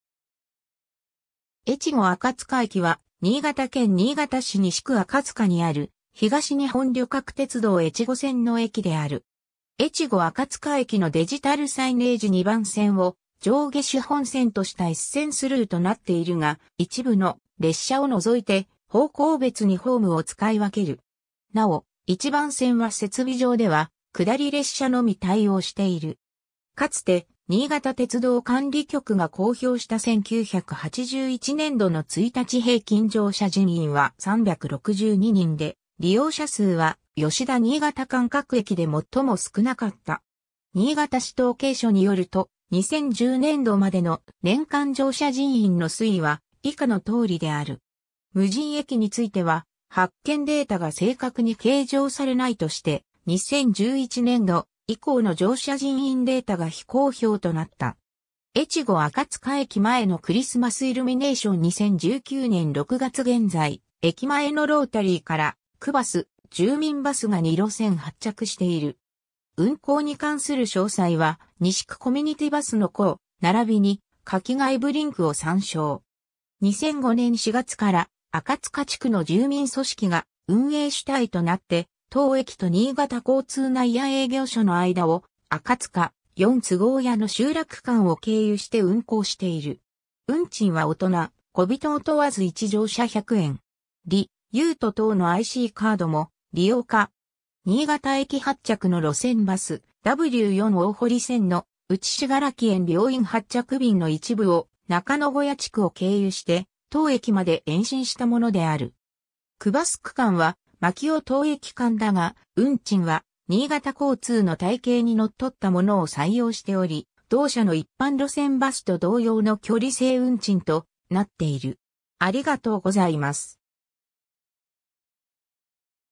越後赤塚駅
越後赤塚駅（えちごあかつかえき）は、新潟県新潟市西区赤塚にある、東日本旅客鉄道（JR東日本）越後線の駅である。
 越後赤塚駅のデジタルサイネージ 2番線を上下主本線とした一線スルーとなっているが、一部の列車を除いて方向別にホームを使い分ける。
なお、1番線は設備上では下り列車のみ対応している。
 かつて新潟鉄道管理局（現在のJR東日本新潟支社の前身）が公表した1981年度（昭和56年度）の1日平均乗車人員は362人で、利用者数は吉田 - 新潟間各駅で最も少なかった。
 「新潟市統計書」によると、2010年度（平成22年度）までの年間乗車人員の推移は以下のとおりである。
 無人駅については、発券データが正確に計上されないとして、2011年度（平成23年度）以降の乗車人員データが非公表となった。
 越後赤塚駅前のクリスマスイルミネーション 2019年6月現在、駅前のロータリーから区バス・住民バスが2路線発着している。
運行に関する詳細は西区#コミュニティバスの項、並びに下記外部リンクを参照。
 2005年4月から赤塚地区の住民組織が運営主体となって、当駅と新潟交通内野営業所の間を、赤塚・四ツ郷屋の集落間を経由して運行している。
運賃は大人・小人を問わず1乗車100円。
「りゅーと」等のICカードも利用可。
 新潟駅発着の路線バス「W4 大堀線」のうち信楽園病院発着便の一部を、中野小屋地区を経由して当駅まで延伸したものである。
区バス区間は槇尾 - 当駅間だが、運賃は新潟交通の体系に則ったものを採用しており、同社の一般路線バス（近郊線など）と同様の距離制運賃となっている。